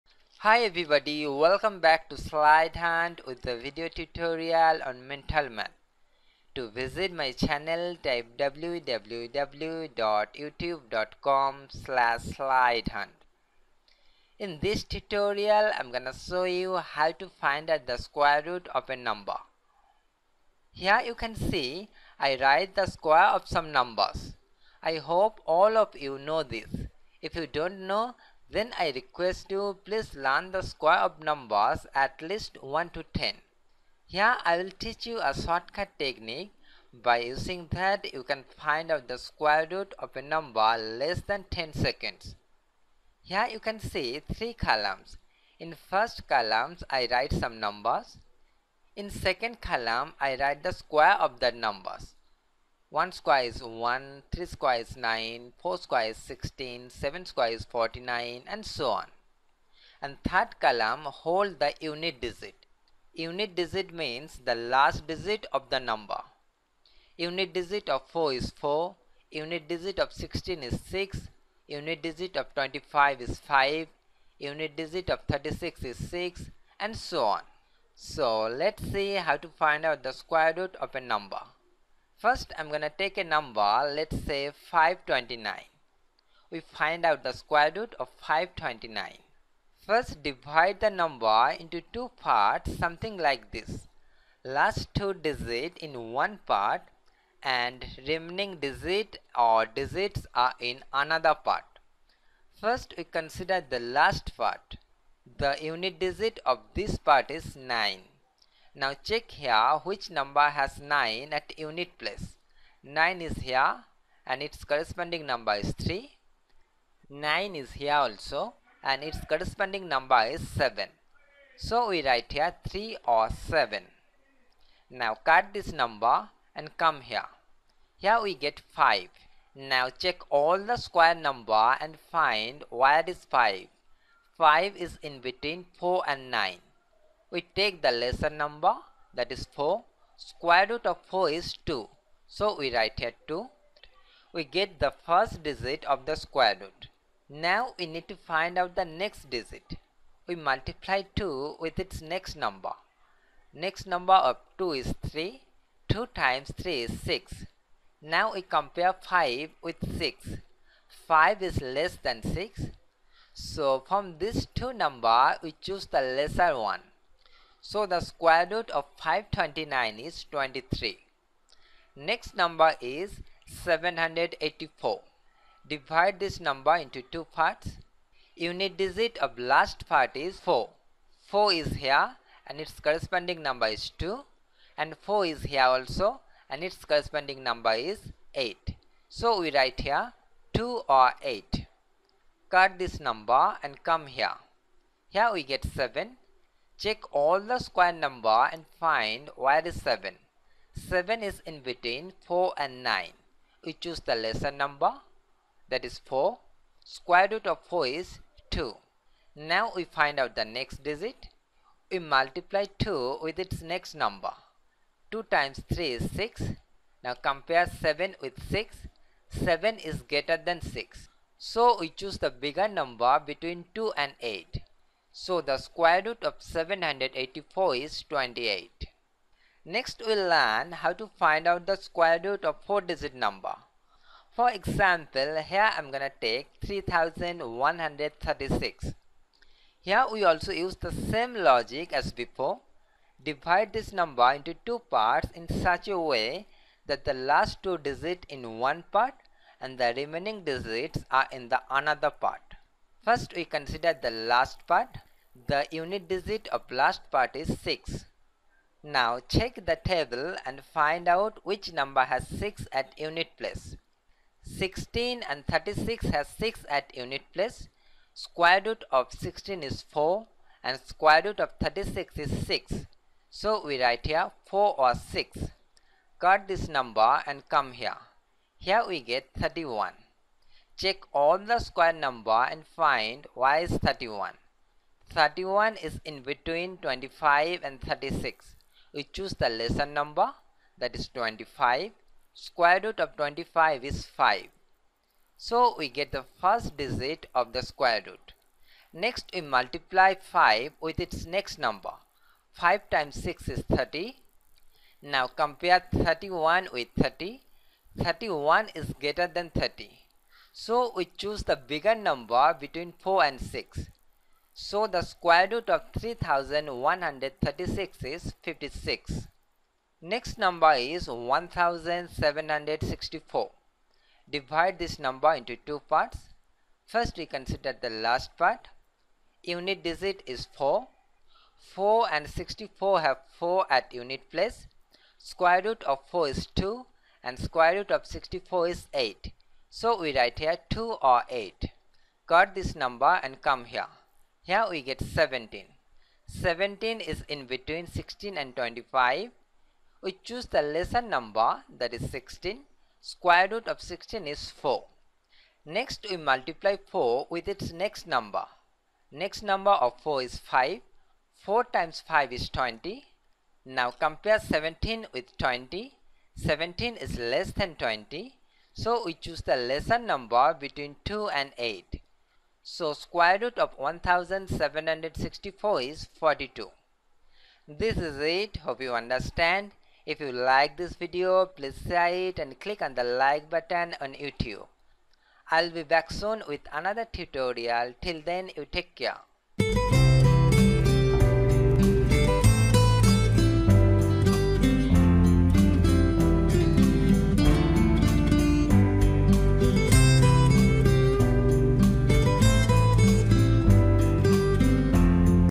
Math trick - Finding the square root of a number
This math trick video will show you how to find the square root of a number. Using this method you can find the square root of a number less than 10 seconds.